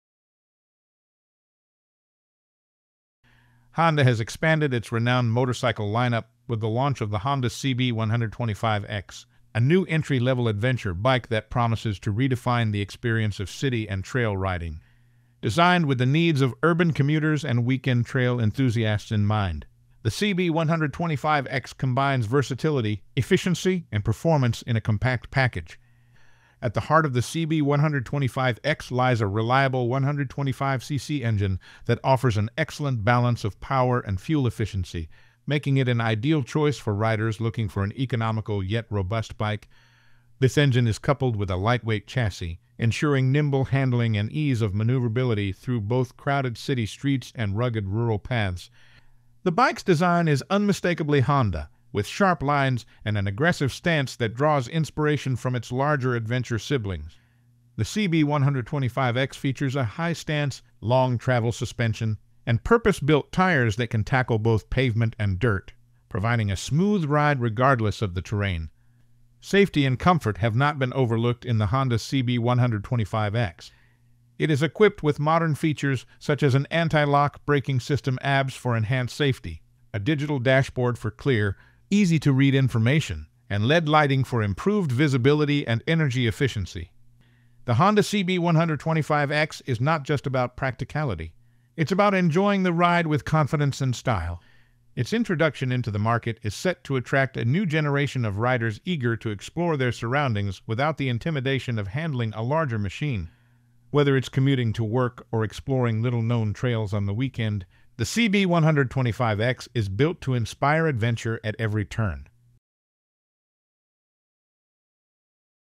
2025 INTRODUCING THE HONDA CB 125 X A FRESH TAKE ON LIGHTWEIGHT ADVENTURE
2025 INTRODUCING THE HONDA CB 125 X A FRESH TAKE ON LIGHTWEIGHT ADVENTURE

Honda has expanded its renowned motorcycle lineup with the launch of the Honda CB 125 X, a new entry-level adventure bike that promises to redefine the experience of city and trail riding. Designed with the needs of urban commuters and weekend trail enthusiasts in mind, the CB 125 X combines versatility, efficiency, and performance in a compact package.